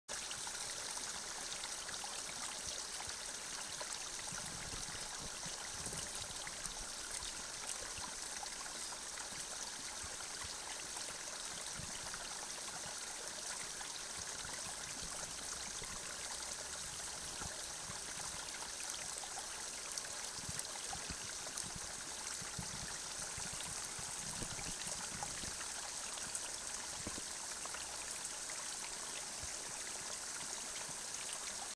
Dragonfly laying eggs
A gold-ringed dragonfly - Cordulegaster boltonii - laying eggs in a shallow stream.